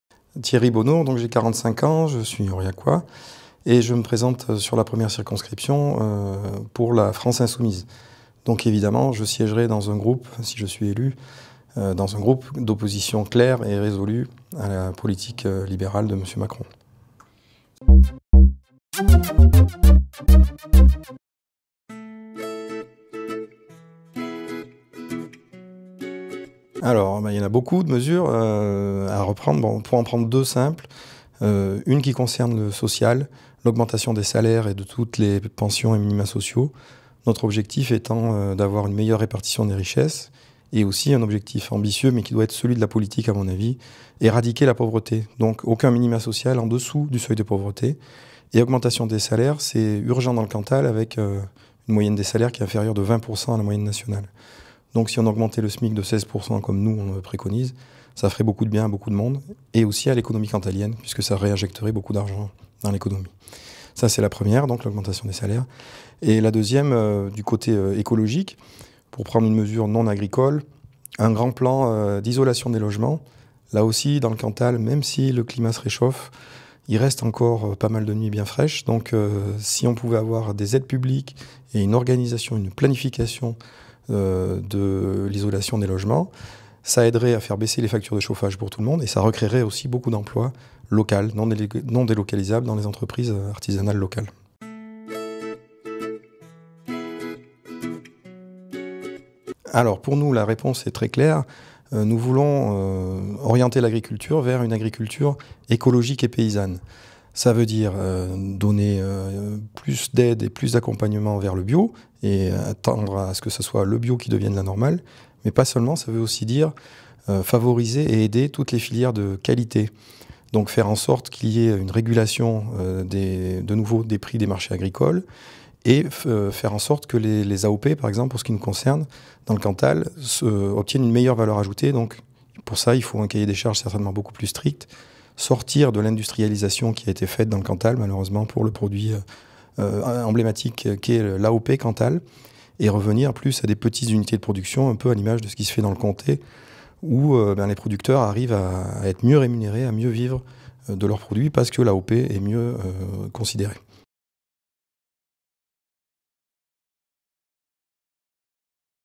Thierry Bonhoure
Interview de Thierry Bonhoure, candidat de la 1ère circonscription du Cantal aux législatives 2017.